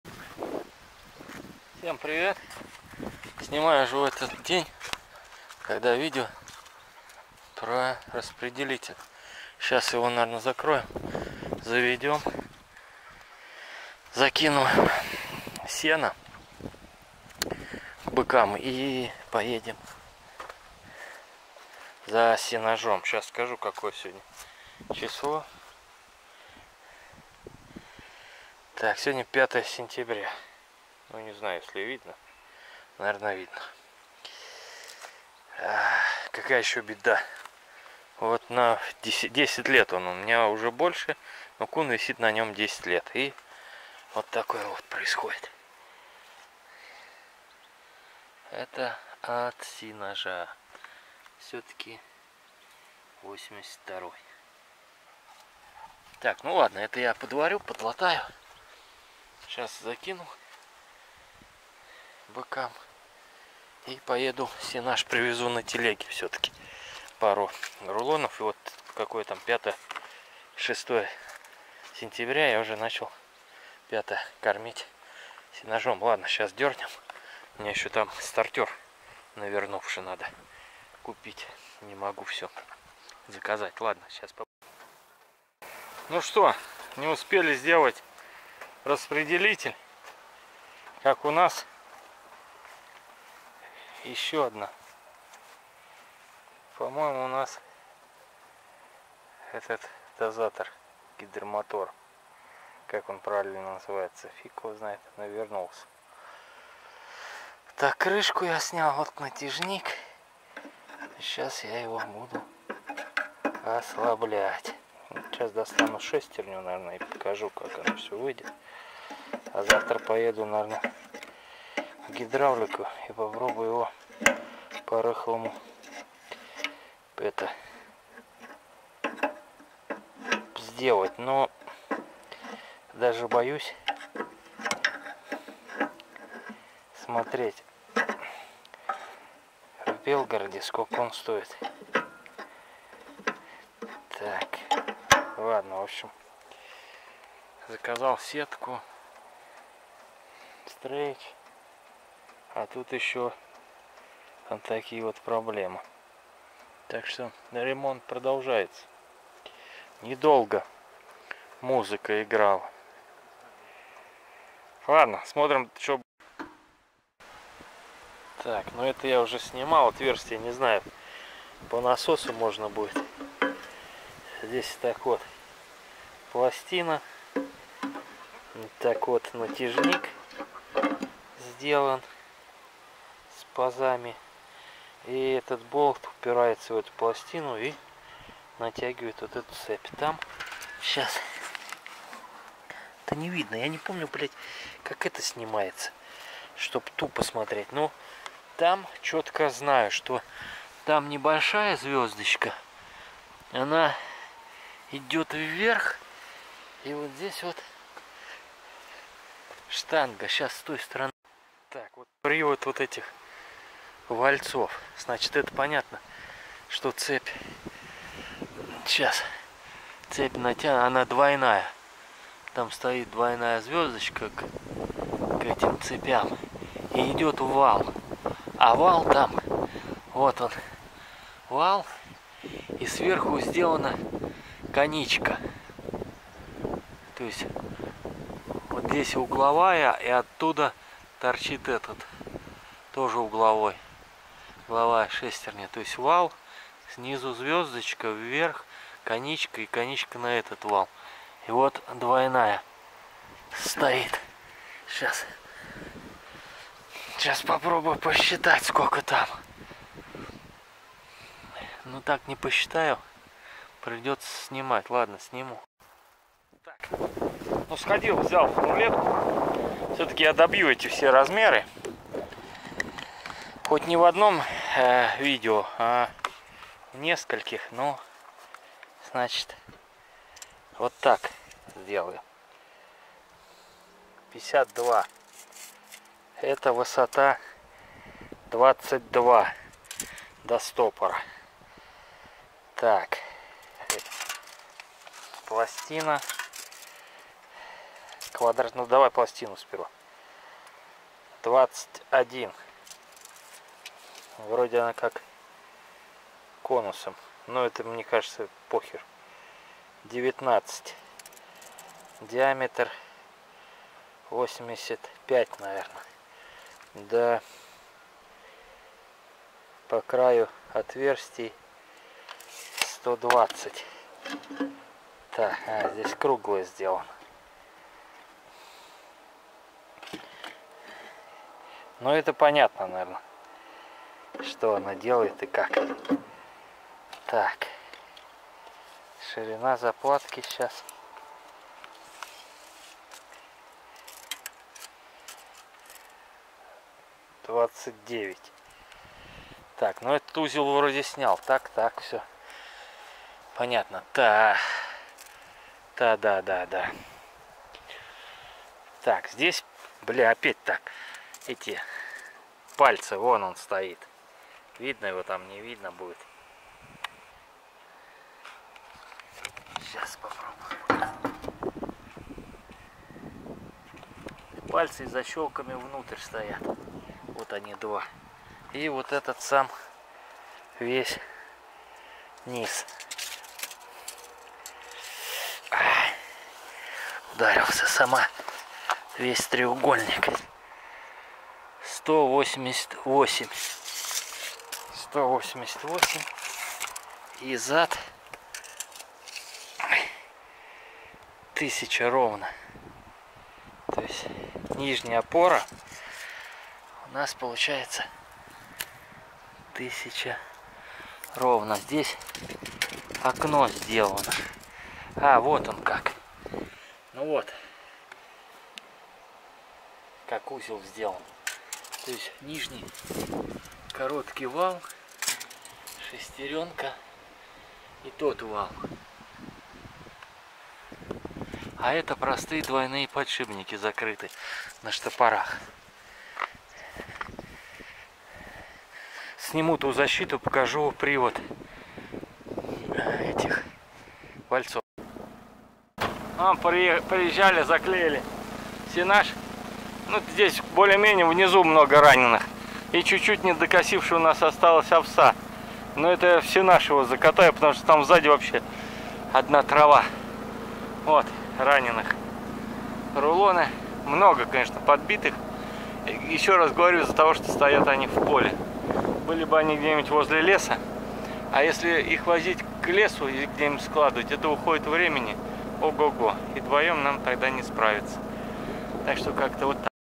Начало ремонтов.сезон 2023.распределитель.обмотчик!
на видео показал-начало ремонтов! тягость и муки поиска причин неисправностей-были решены!